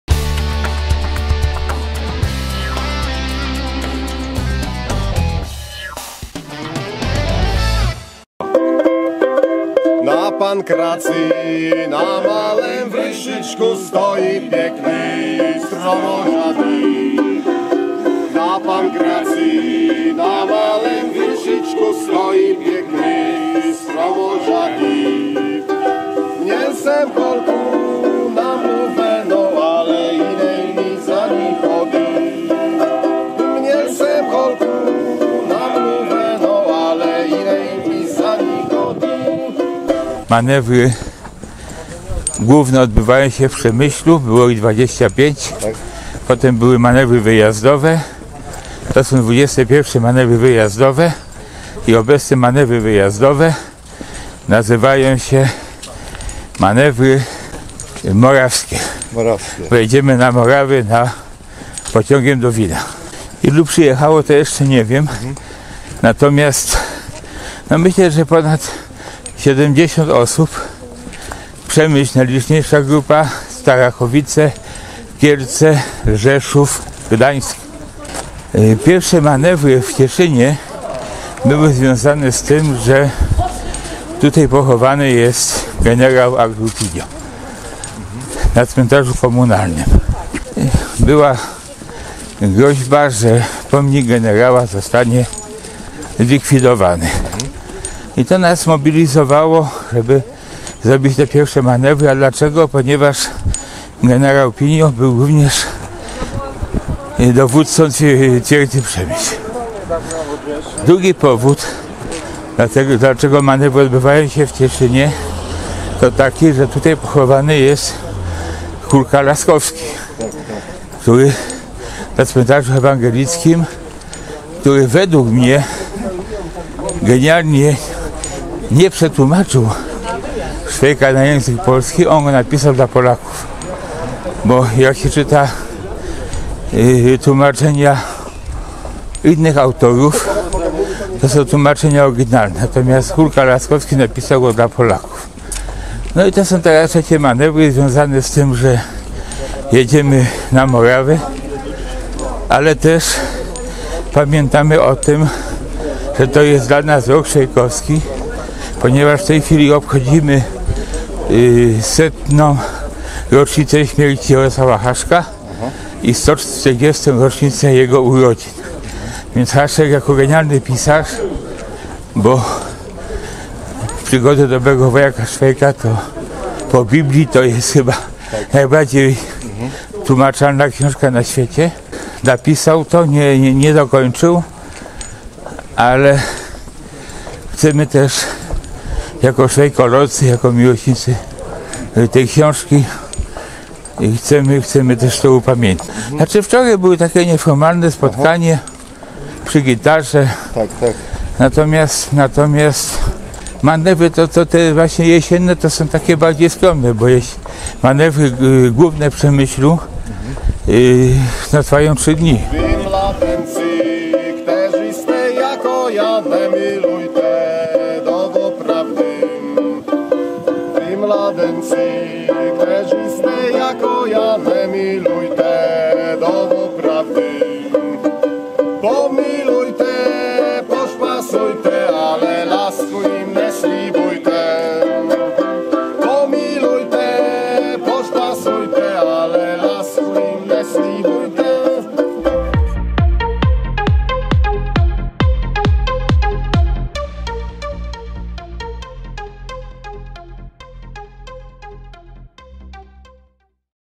Jesienne Manewry Szwejkowskie zorganizowało Przemyskie Stowarzyszenie Przyjaciół Dobrego Wojaka
Od piątku (06.10) w Cieszynie trwają
Jesienne Manewry Szwejkowskie (Morawskie), które zorganizowało Przemyskie Stowarzyszenie Przyjaciół
Dobrego Wojaka Szwejka. Była to XXI edycja Jesiennych Manewrów. Miłośnicy Dobrego
Wojaka Szwejka do Cieszyna przyjechali po raz trzeci i w sobotę uczestniczyli w wycieczce pociągiem
na coroczne winobranie w Valticach.